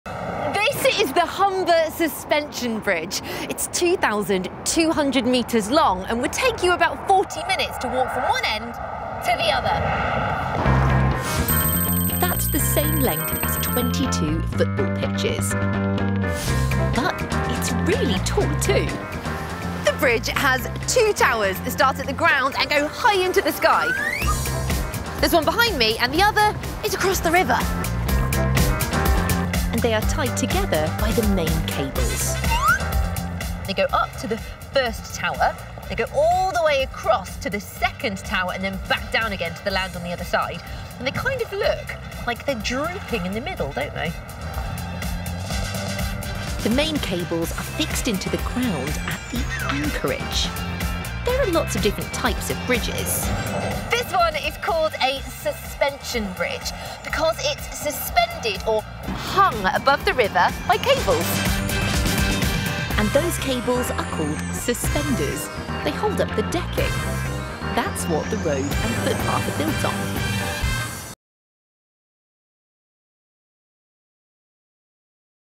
How Do Bridges Work? 🌉
Maddie learns how a suspension bridge works.